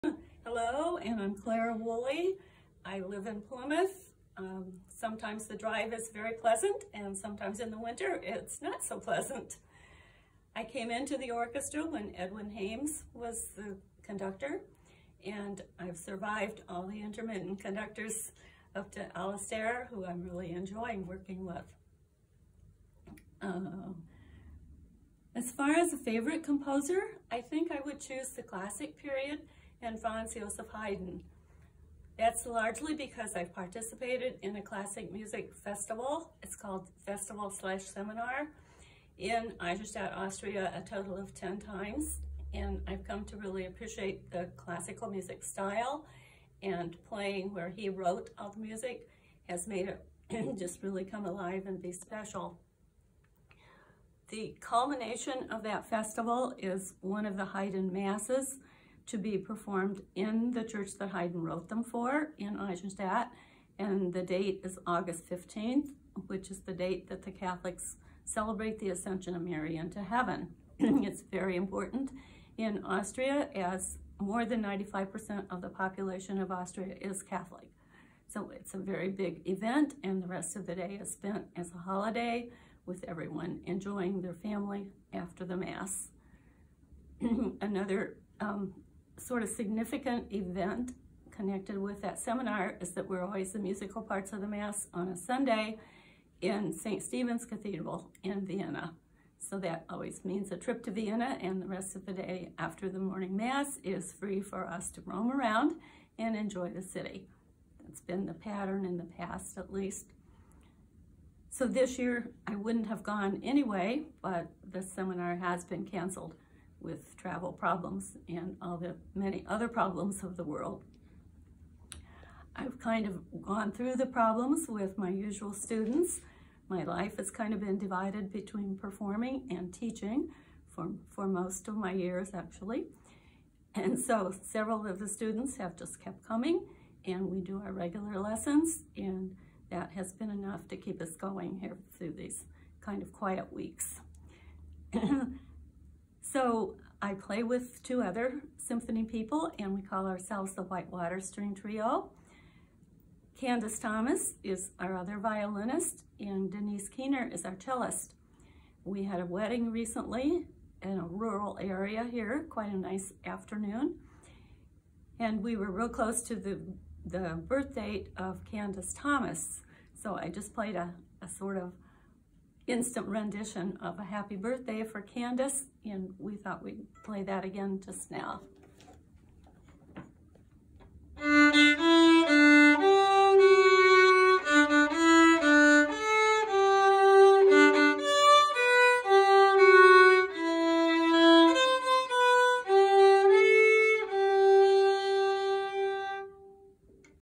Musicians Monday, August 10, 2020 – Clara Woolley
Clara Woolley joined the South Bend Symphony when Edwyn Hames was the music director. She shares her favorite composer and birthday wish for a fellow SBSO musician. 🎂